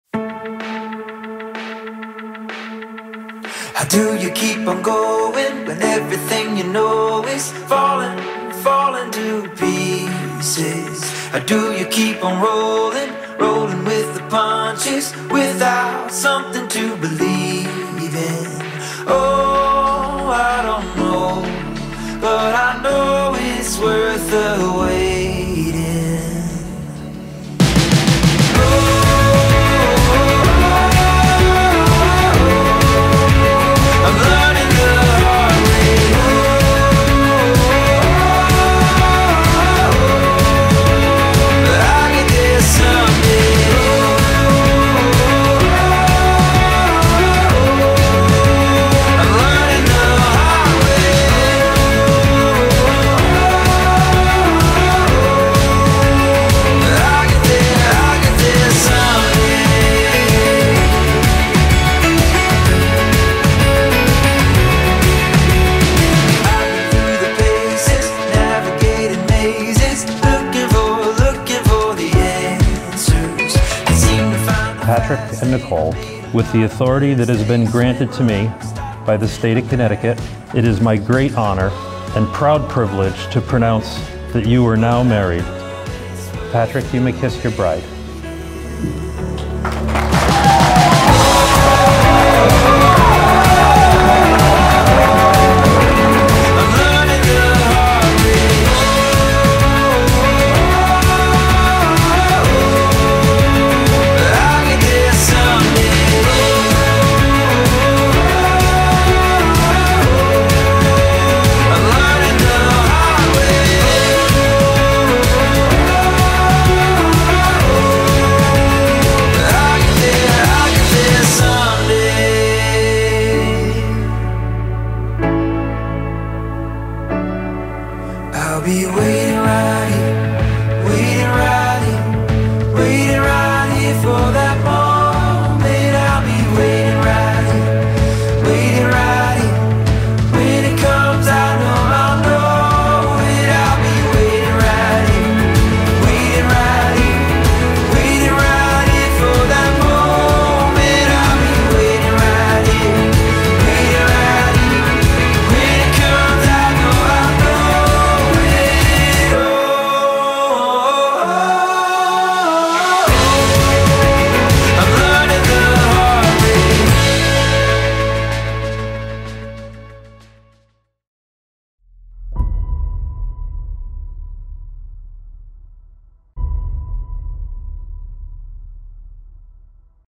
Pat and Nichole 9.29.2023
An Early Summers Dream Wedding
Gorgeous Venue, Gorgeous Couple, Amazing Entertainment Team, Congratulations to this amazing couple!

Video Shot at Waters Edge Westbrook CT

The Go Event Group
Video by Go DJ Entertainment, Go Video, Go Photo, Go Entertainment Group. Farmington CT USA 06032.

Not a copyright "__"

Song